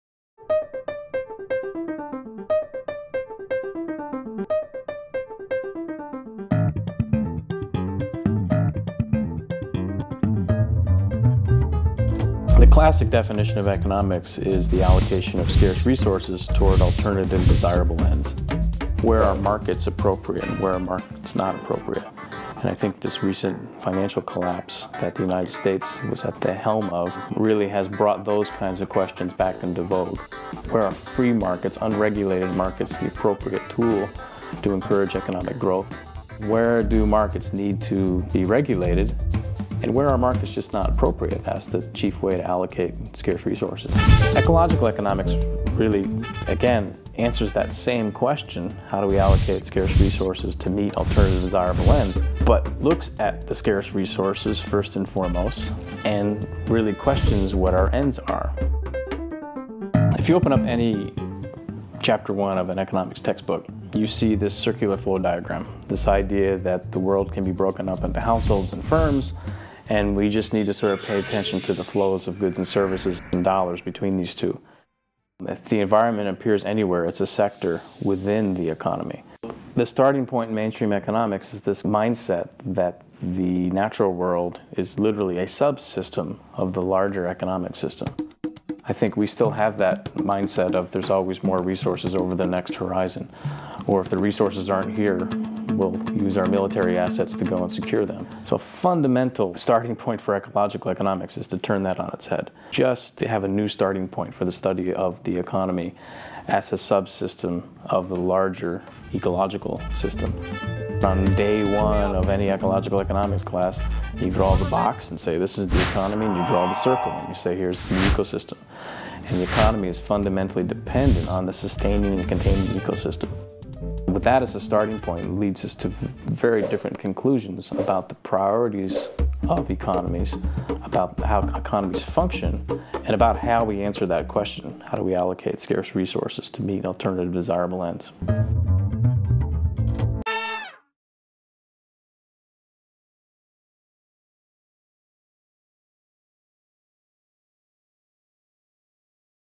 Jon Erickson on Ecological Economics
Jon Erickson, Managing Director of the Gund Institute for Ecological Economics at the University of Vermont, examines how we view the environment in conjunction with our economic system.